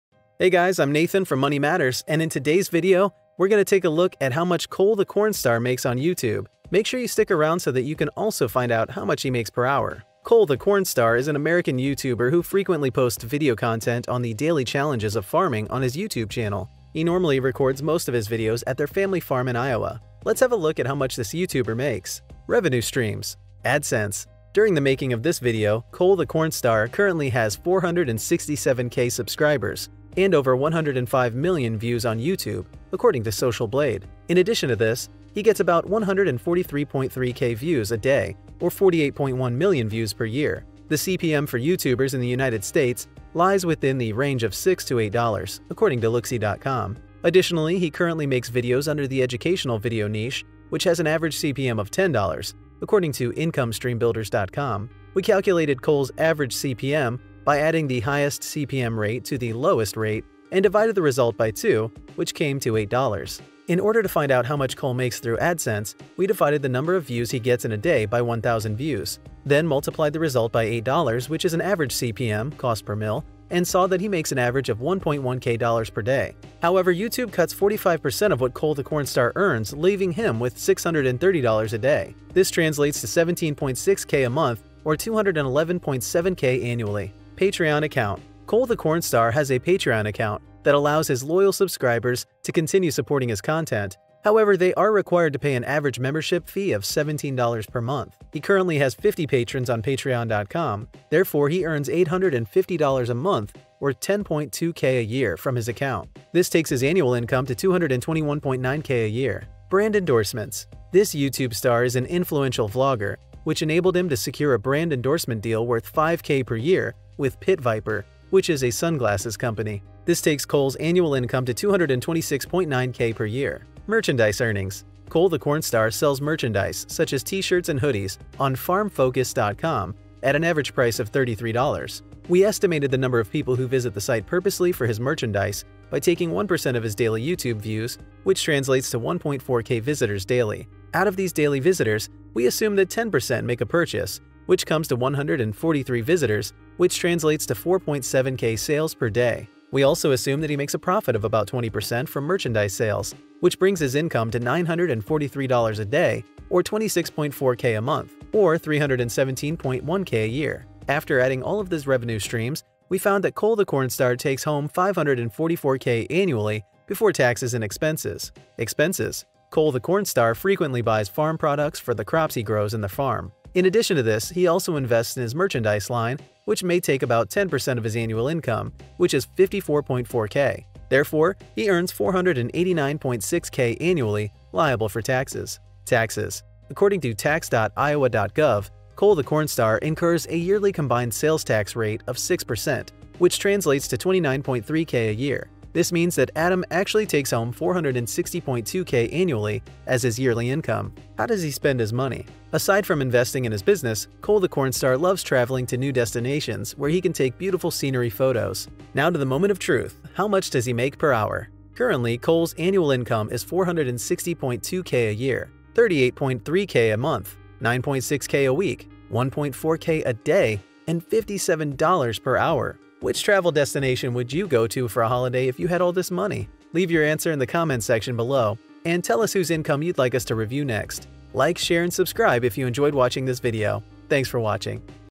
The YouTube Income of Cole the Cornstar Unveiled (Take a look inside)
Cole the Cornstar is an American YouTuber who frequently posts video content on the daily challenges of farming on his YouTube channel. He normally records most of his videos at their family farm in Iowa.

Let’s have a look at how much they make.

AdSense -  We estimate that Cole the Cornstar currently makes $630 every single day

Merchandise earnings - Assuming he makes a profit margin of 20% from the merchandise sales, their income would be an estimated $1.5k a day, $45.3k a month, or $ $544k  per year.

Brand endorsements - Cole the Cornstar has managed to secure brand endorsement deals worth approximately $5k a year.

Expenses
Cole the Cornstar uses approximately 10% of their annual income to invest in their merchandise line and purchase farm products which adds up to $54.4k leaving $489.6k liable for taxes.

Taxes
They incur a yearly combined sales tax rate of 6% which is $29.3k. This means that they take home $460.2k per year.